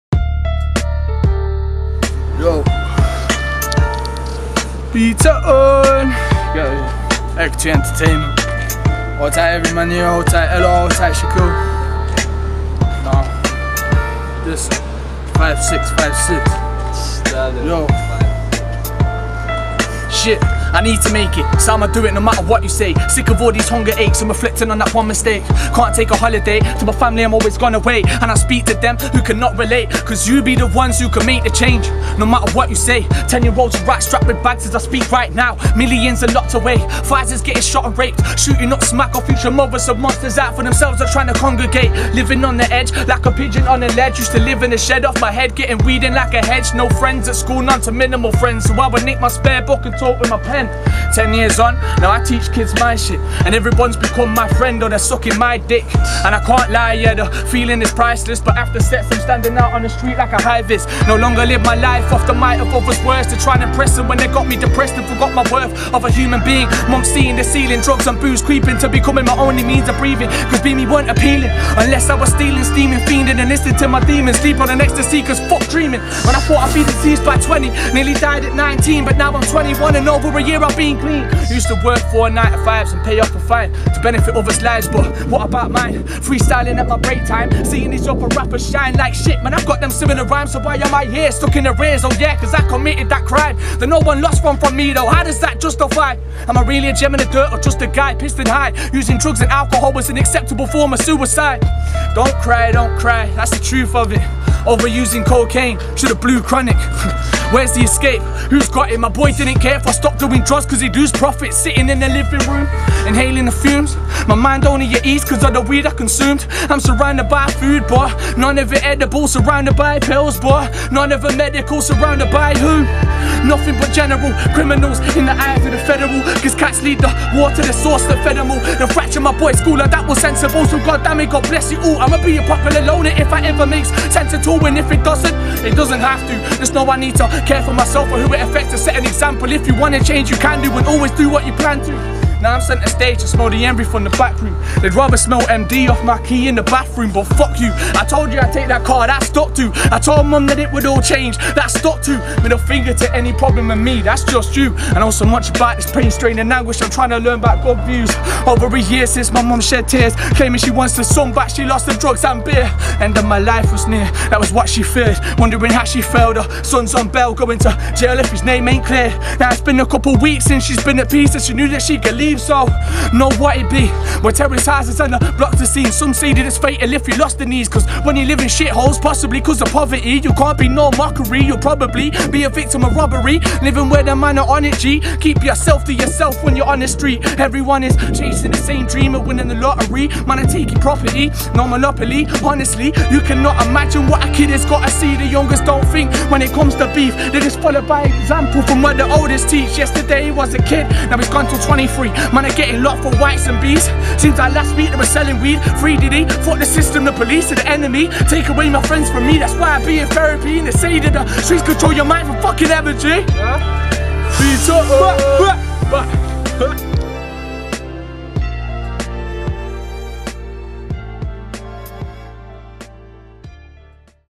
HIVE TV: BEETONE | OFF THE TOP
We're back with another Off the top session, and this week we've got Derby's Beetone.

HIVE TV Also run a